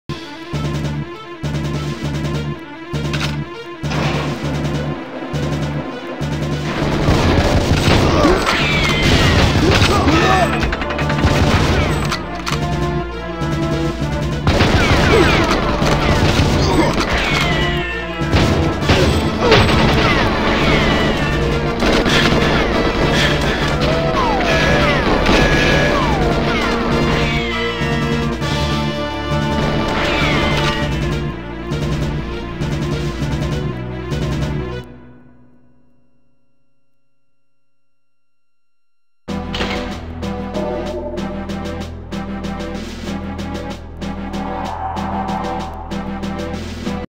speedruntrainer - Czech Temple Secret Agent 0:28
same as on Agent with an extra objecive
mainframe warp cuts almost the whole level. Enjoy